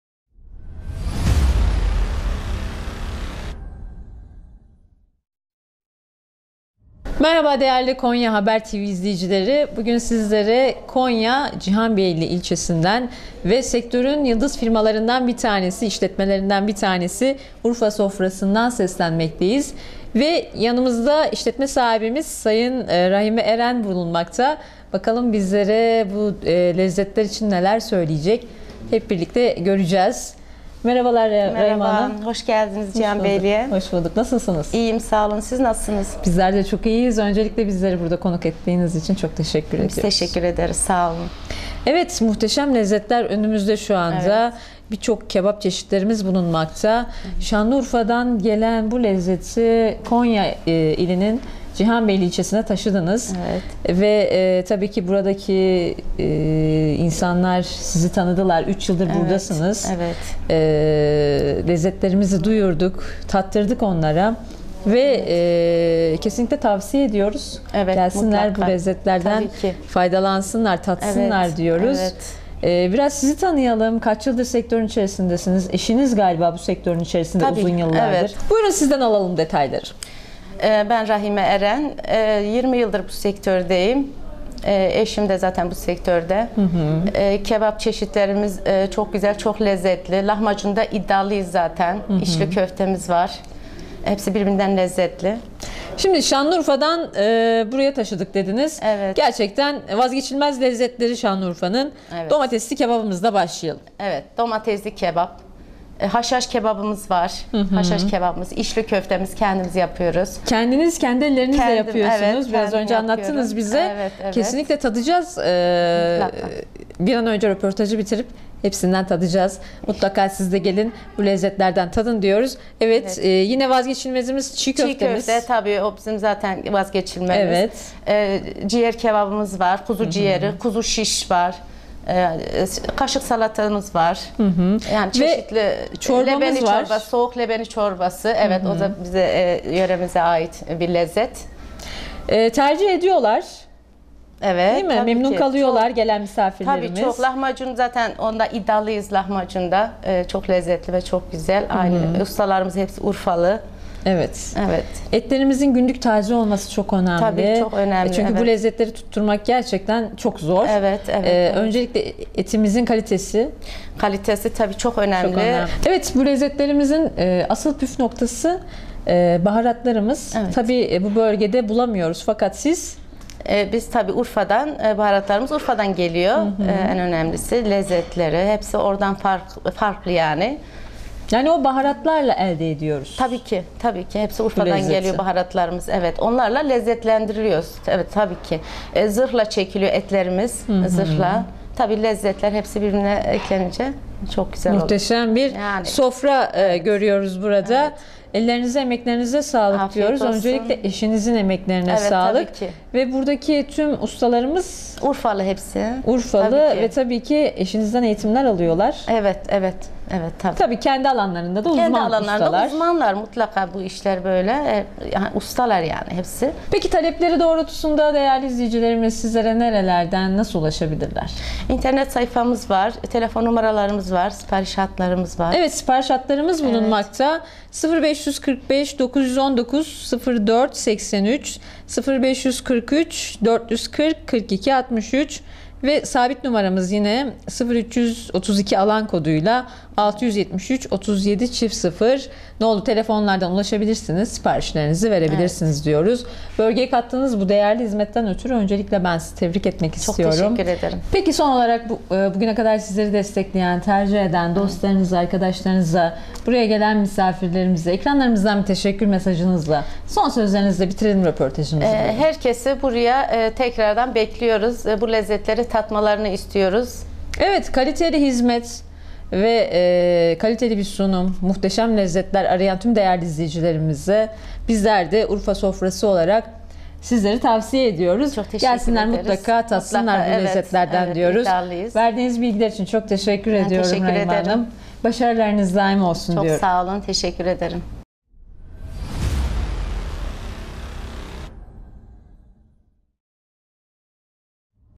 URFA SOFRASI - CİHANBEYLİ/KONYA
URFA SOFRASI  firma sahibi Rahime EREN, başarı ve faaliyetlerini Konya Haber Tv muhabirimiz Zuhal TARIMER' e anlattı.